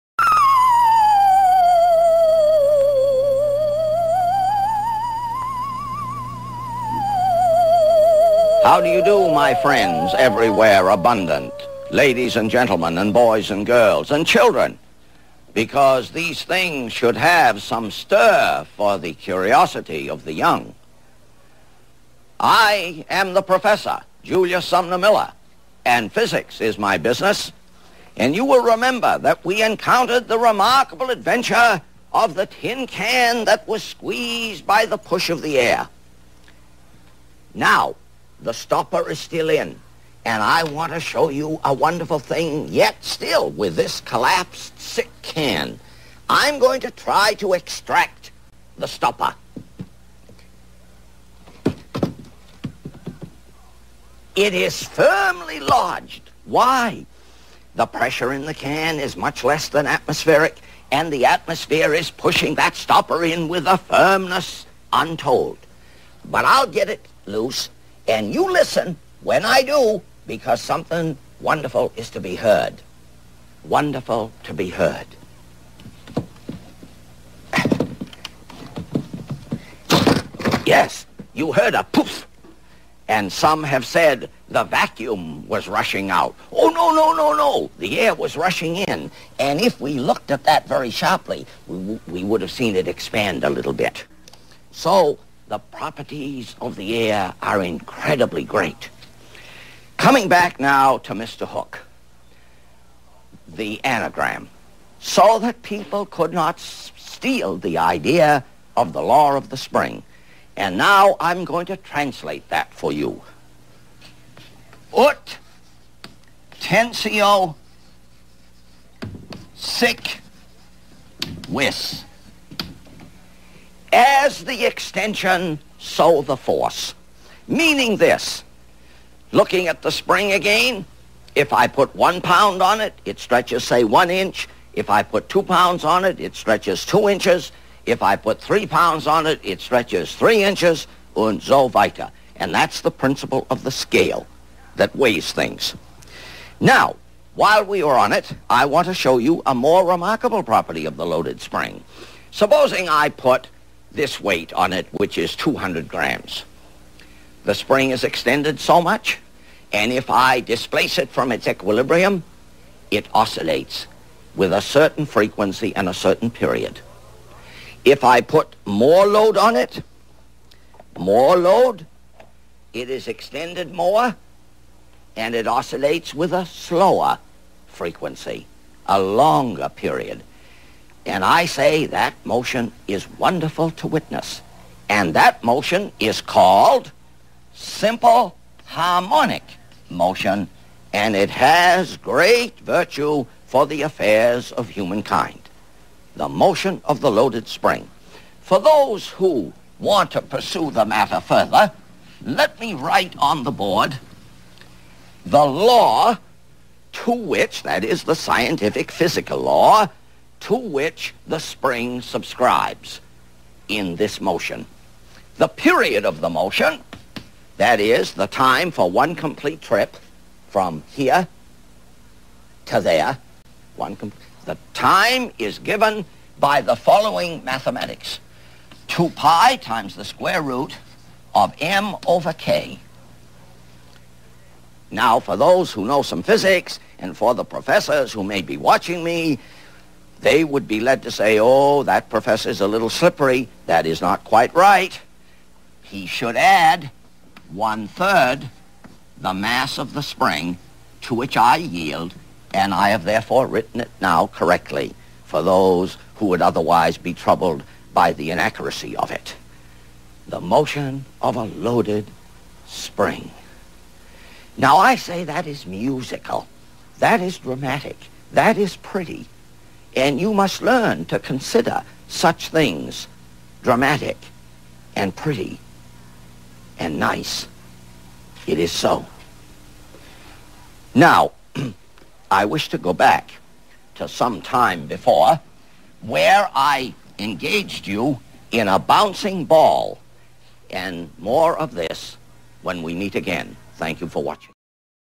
Julius Sumner Miller 62 HHOF - Atmospheric Pressure, Hooke's Law Cont., Simple Harmonic Motion
Professor Julius Sumner Miller demonstrates that the pressure from the atmosphere can hold the lid tighter on a container whose air has been evacuated than one whose air has not.  He decodes the anagram presented in the last video, and gives us the formula for the period of a spring demonstrating simple harmonic motion.